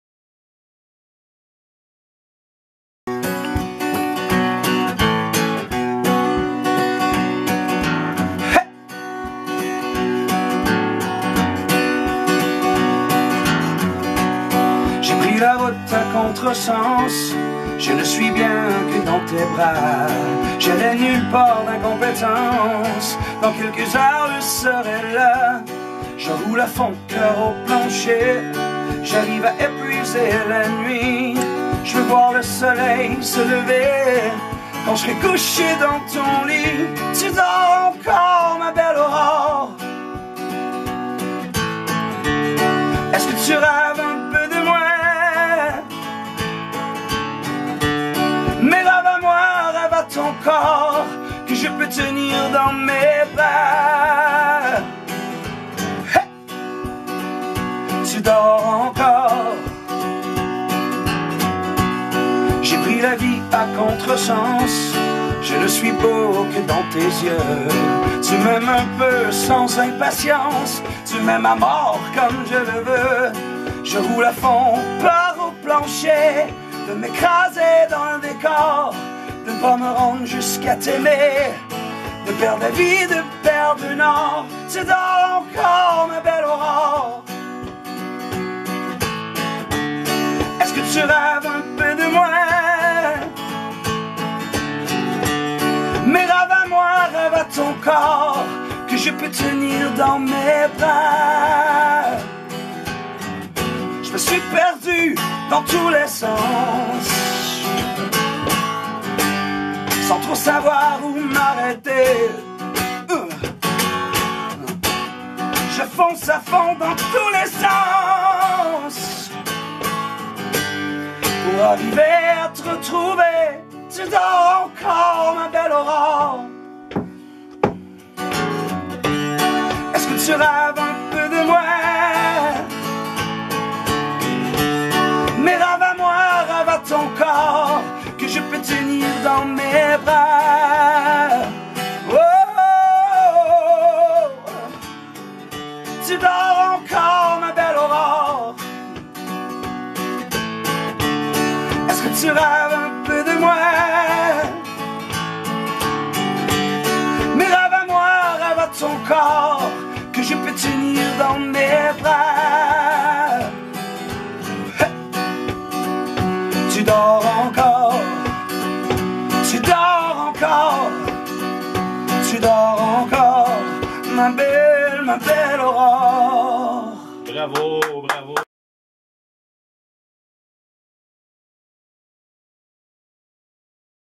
Mathieu Provençal
Mathieu Provençal dans les studios du M103,5 le 17 mars 2016